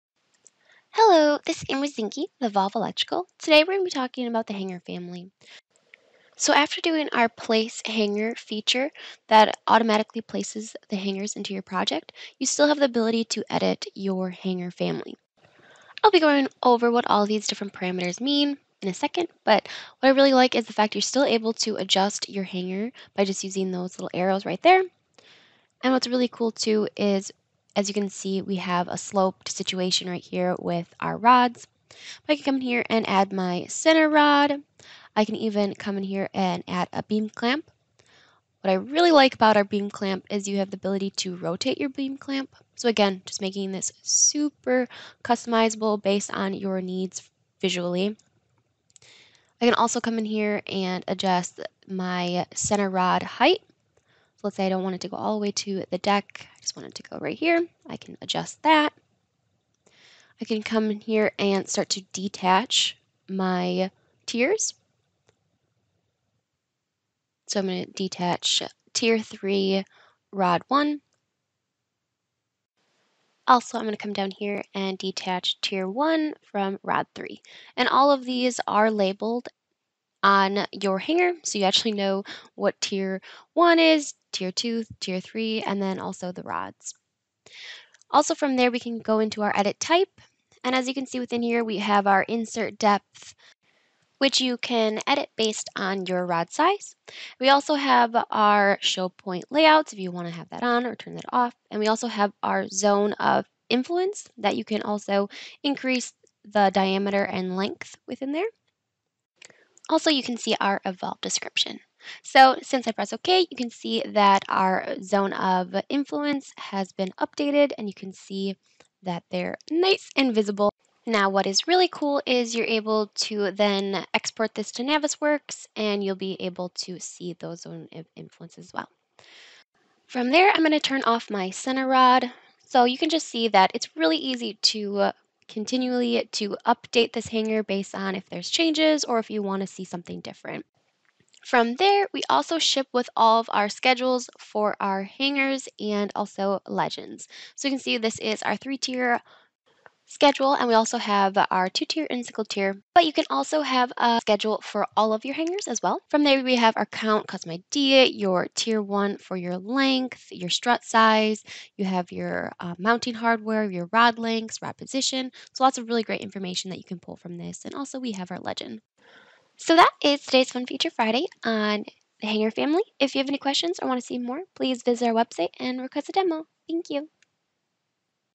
eVolve Electrical - Hanger Family
After doing the place hanger feature, all of your hangers will be automatically placed, but you still have the ability to adjust the hanger after the fact. You can detach tiers, add center rods, adjust rod lengths, add hanger attachments, rotate beam clamps, adjust insert depth values, show zone of influence, and many more. Schedules and legends for hangers are shipped with eVolve Electrical.

▬ Social Media  ▬▬▬▬▬▬▬▬▬▬▬▬